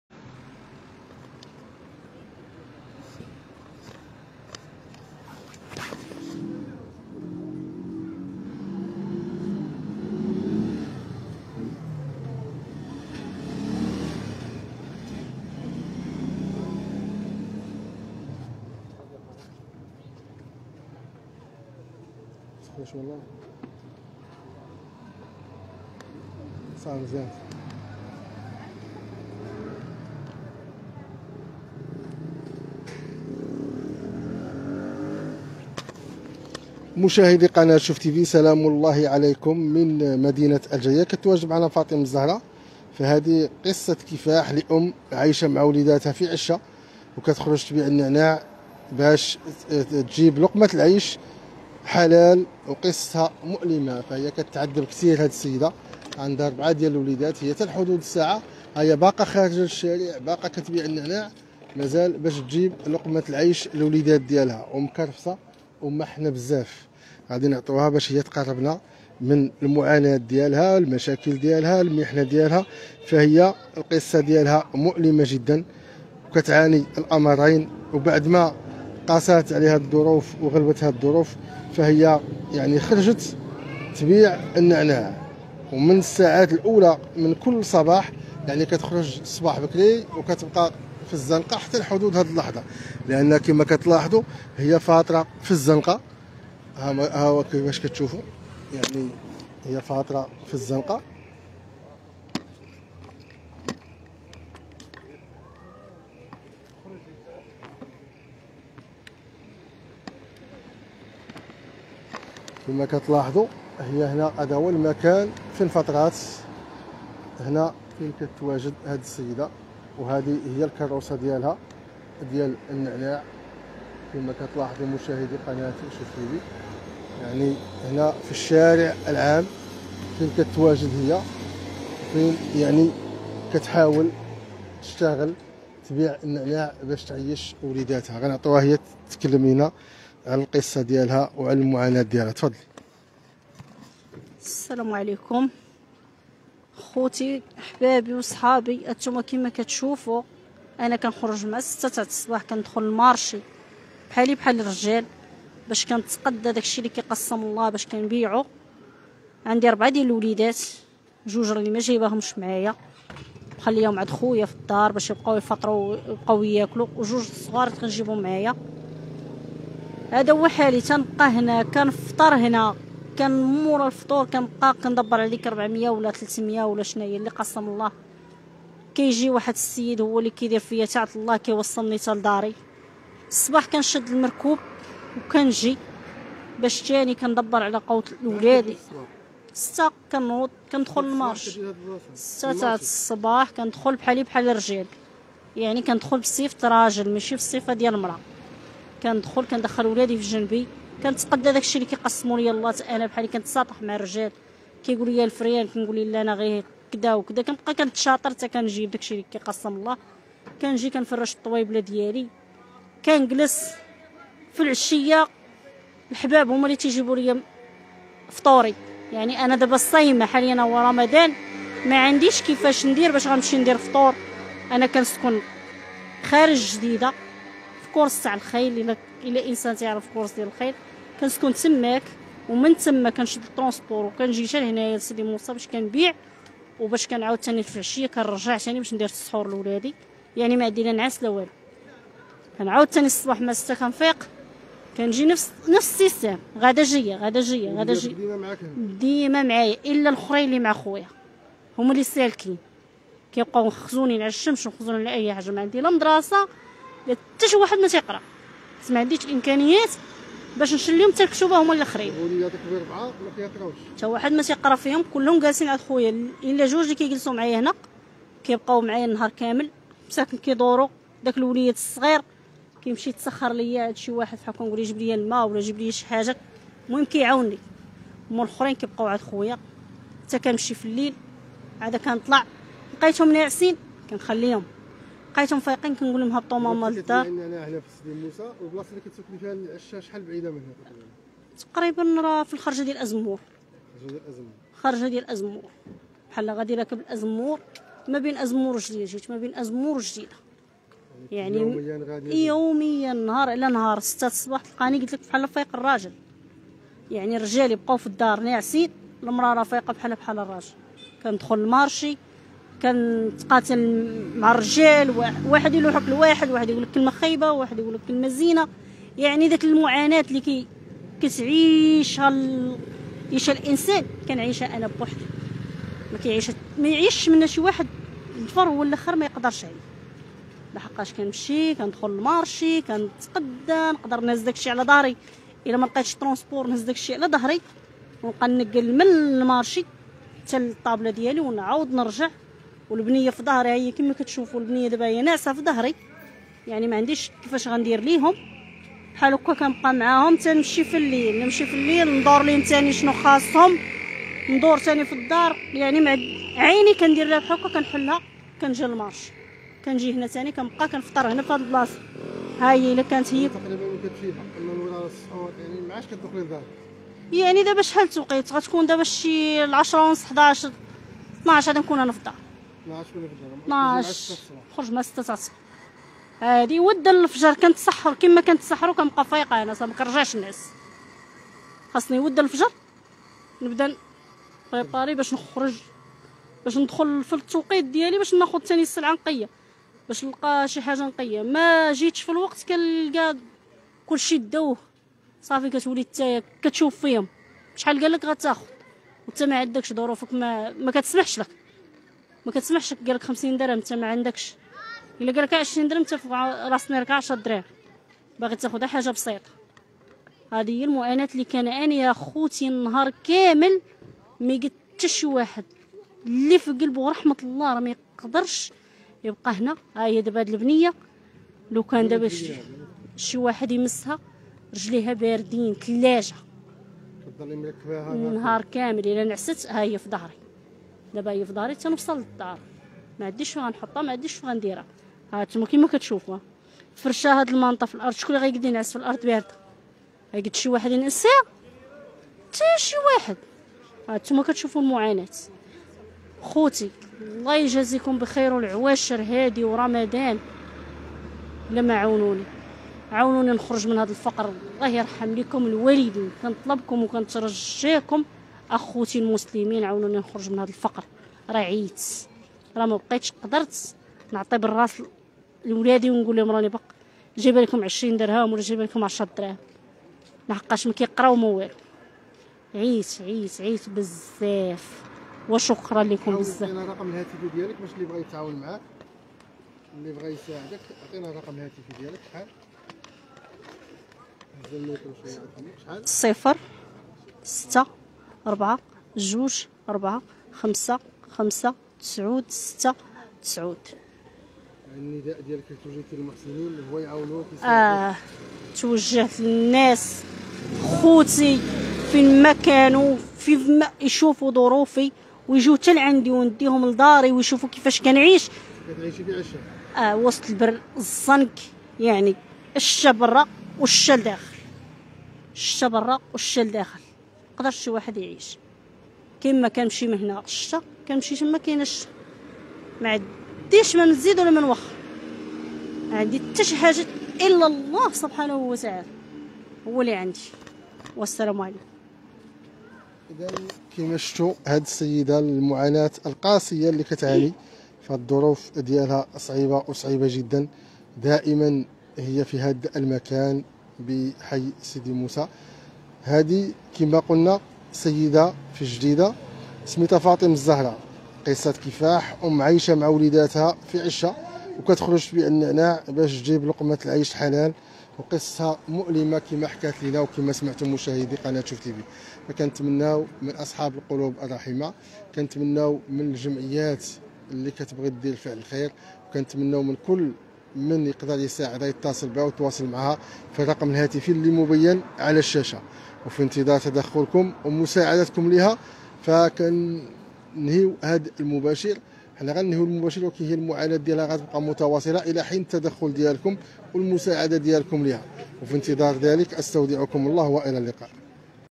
قصة كفاح أم عايشة مع وليداتها فعشة وكتخرج تبيع النعناع باش تجيب لقمة عيش حلال وقصتها مؤلمة
مباشرة من الجديدة:"فاطمة الزهراء"قصة كفاح أم عايشة مع وليداتها فعشة وكتخرج تبيع النعناع باش تجيب لقمة عيش حلال وقصتها مؤلمة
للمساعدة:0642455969